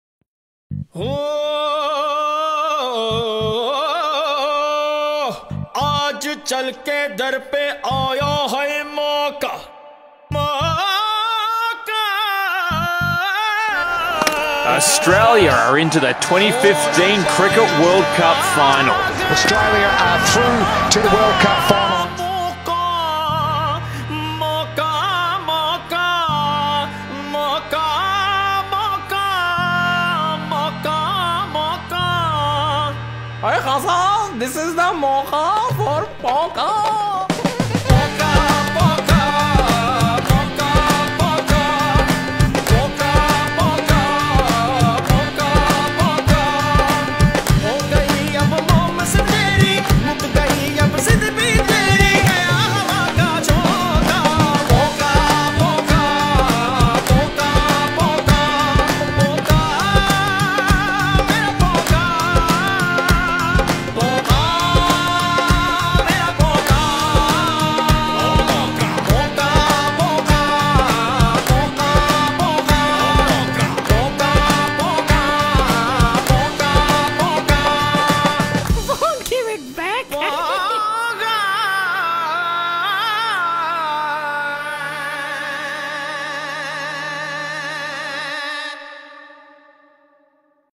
A fun & fitting reply to starsports MaukaMauka | Ponka
A fitting reply from across the border to India's MaukaMauka TV ad campaign to humiliate Pakistan! This video is intended just for fun, no hate no diss! We spread love and spread.
Peace.
Like the video for better reach!